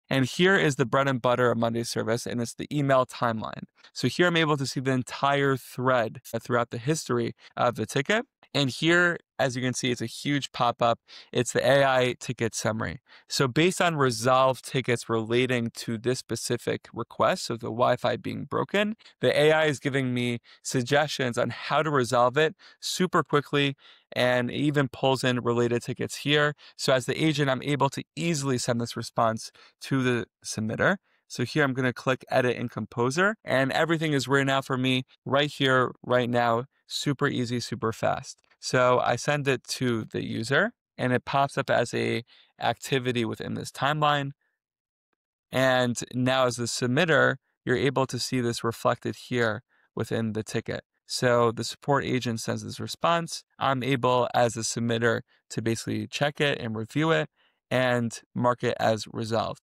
Watch AI Handle Customer Support Like Magic! (with Jack from monday.com)
Jack Stepner, Technical Support Specialist at monday.com, gives a behind-the-scenes look at the email timeline inside monday service — the core of how agents manage and resolve tickets.

Watch how Jack:
✔ Reviews the full email thread for the ticket
✔ Uses the AI Ticket Summary to get fast suggestions
✔ Pulls in related tickets automatically
✔ Sends a ready-to-go reply using the composer
✔ Shows how end users can see updates and mark the ticket as resolved

It’s the fastest way for support agents and submitters to stay perfectly aligned — all inside monday service. ⚡️

*FREE RESOURCES*

*RELATED SEARCHES*